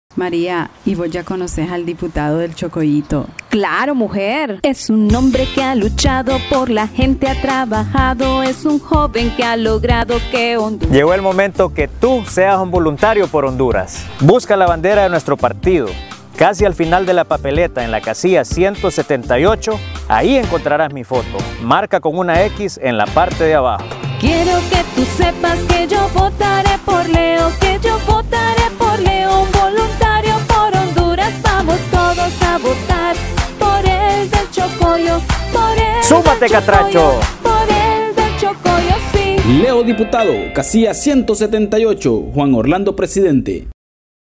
Spot tv Leo Diputado el del Chocoyito
Vamos todos a votar por el del Chocoyo por el del Chochoyo si..! Leo Castellon Diputado por Cortes Casilla 178 Juan Orlando Presidente.